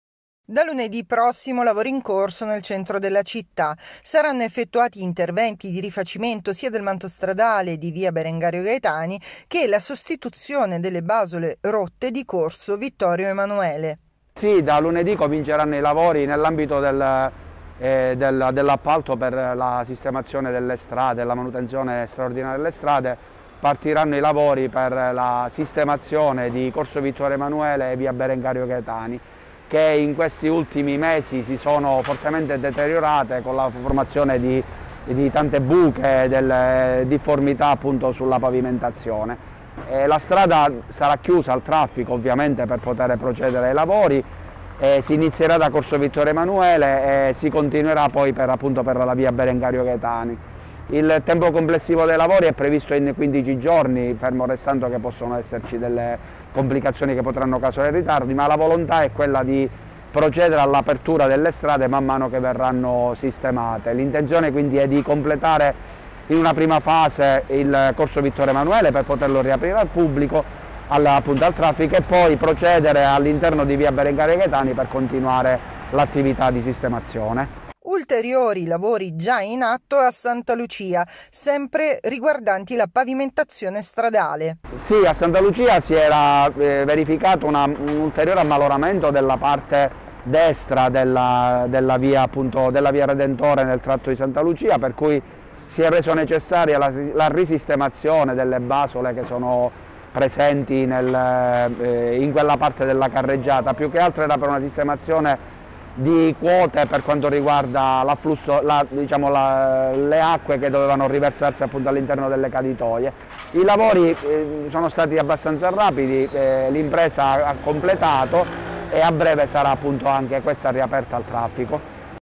Caltanissetta - Chiuse per lavori due strade del centro storico - TFNWEB.IT
Da lunedì corso Vittorio Emanuele e via Berengario Gaetani saranno interdetti alle auto per la sostituzione delle basole. I lavori dureranno 15 giorni